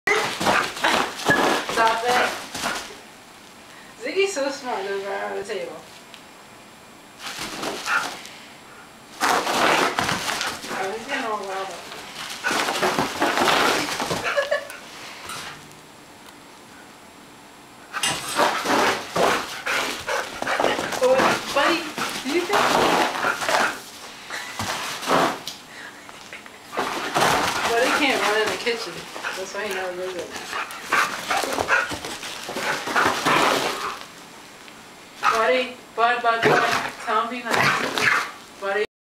ziggy meatball and buddy having some fun!!
boston terrier and chow mix chasing each other like retards lol!!